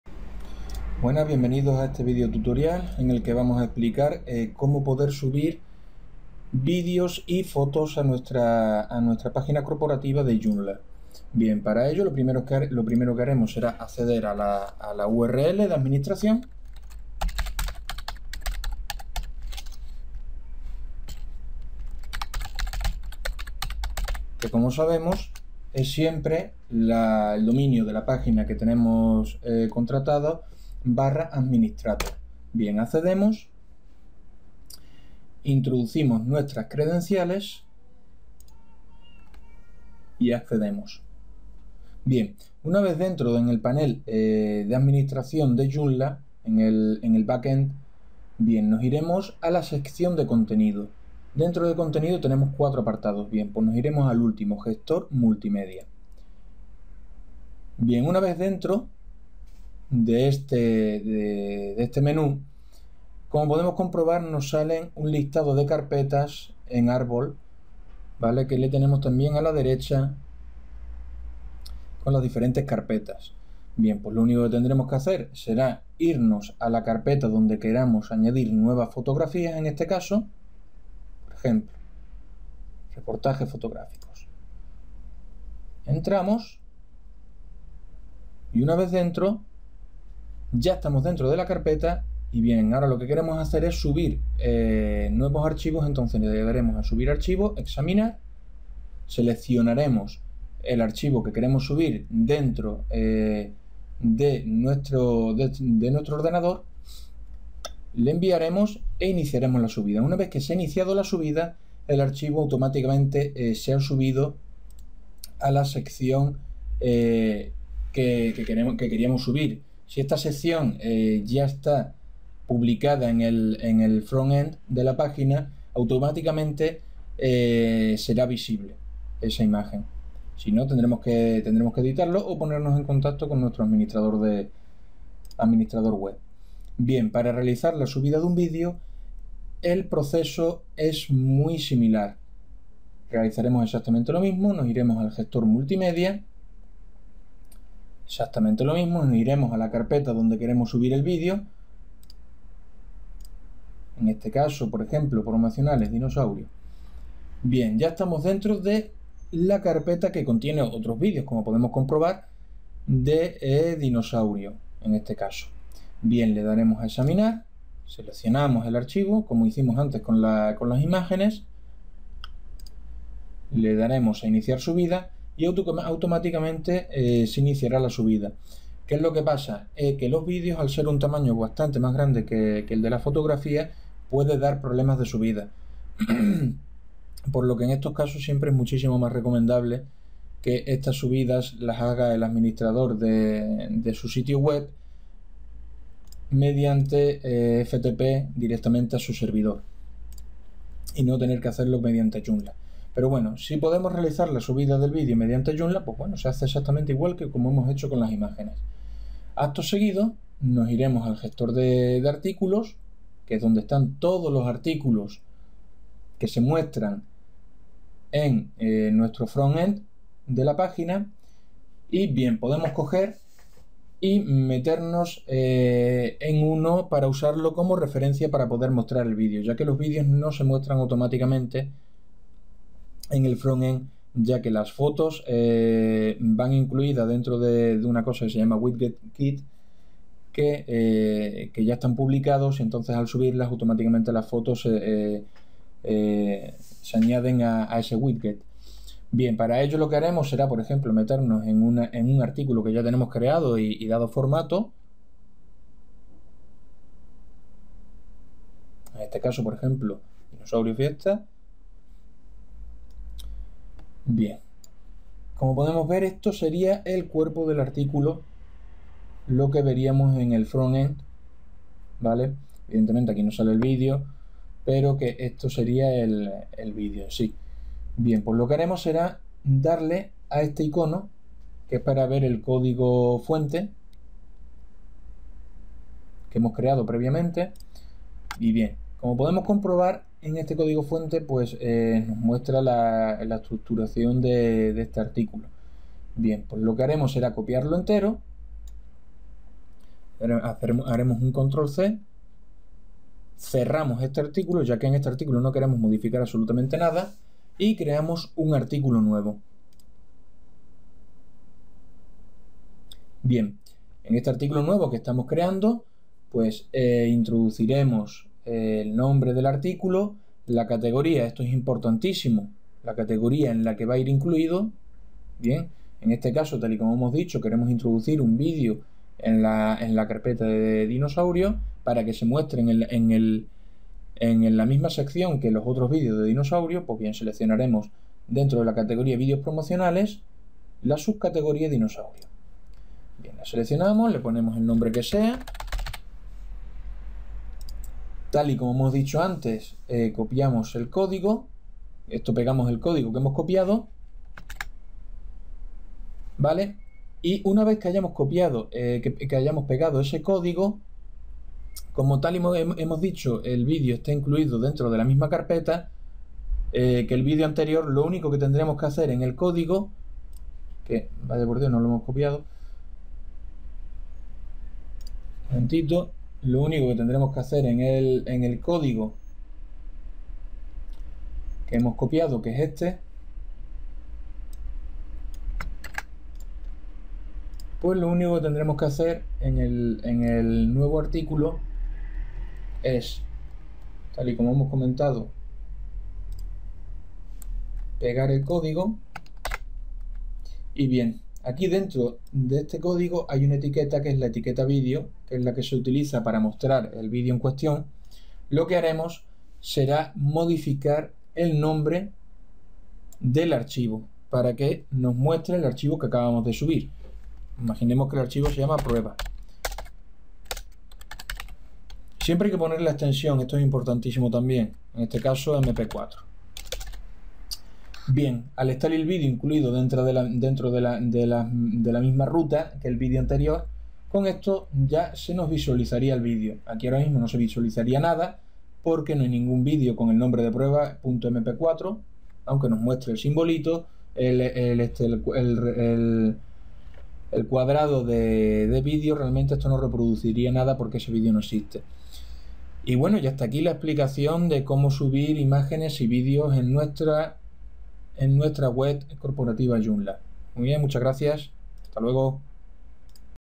Subir imagenes y videos Joomla
Tutorial sencillo y rapido de como subir nuestras imagenes y videos en Joomla